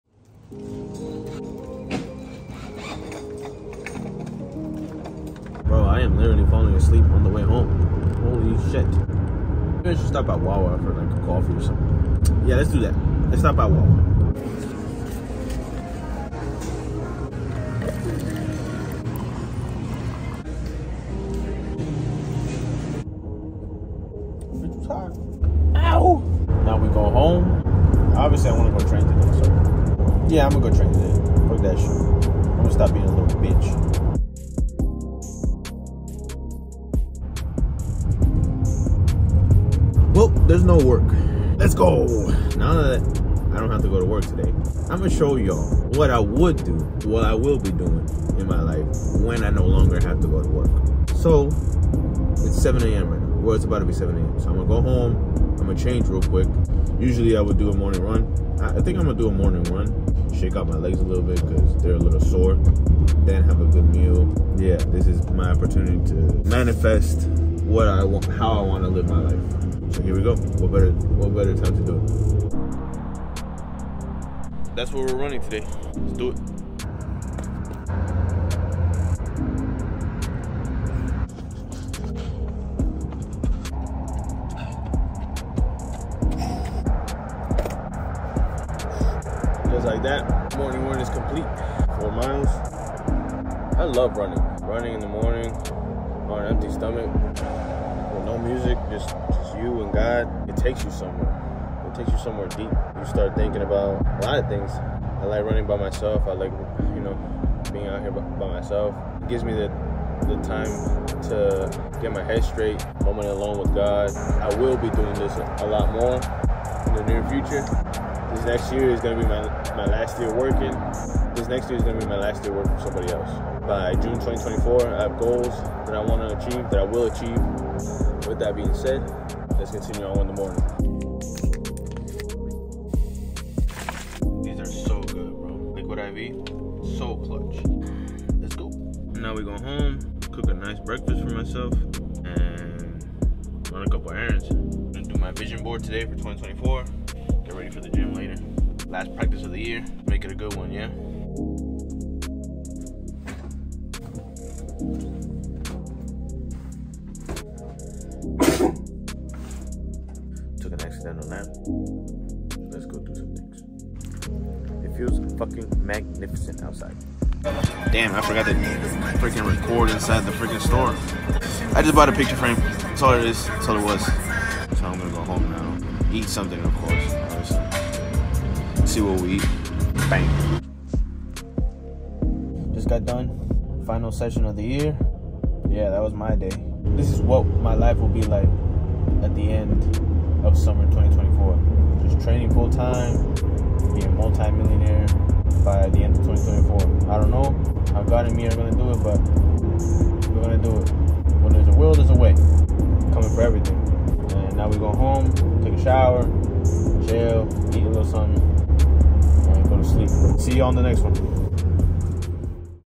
Just Decide | Day in the Life Vlog
These are a few days in my life where work is super slow so I can go home and actually train and do what I want to do without my body being broken down completely. I cherish these moments because I use them to visualize what I want my life to look like.